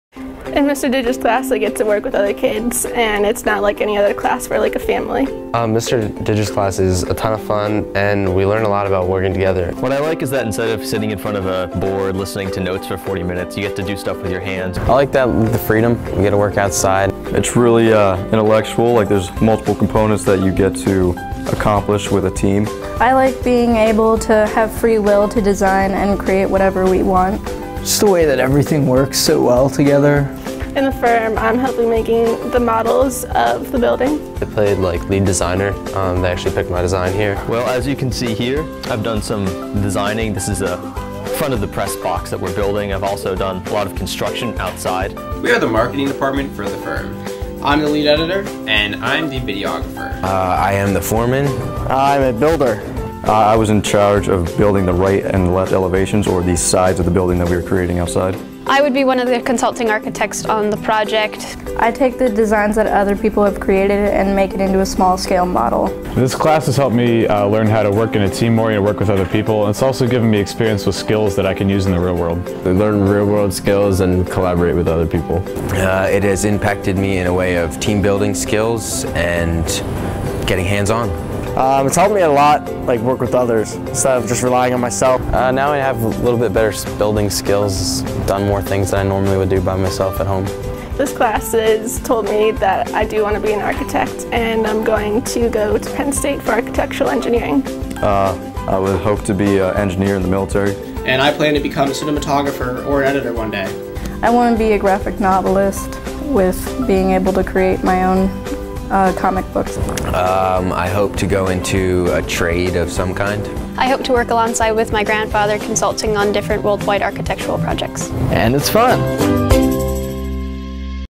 Emmaus High School Architecture class
Emmaus High School teacher Mr. Scott Didra has created an "office like" learning environment in his Architecture classroom where students can experience what it is like to be in a firm.  Students collaborate ideas as they work with clients to design real world projects.  The class chooses a winning design and then works together to build the project in a real world setting. This is a short 2 minute video of students expressing how they feel about being a part of "The Firm".  The video was produced and edited by students Elliot Laubach and David Pickett.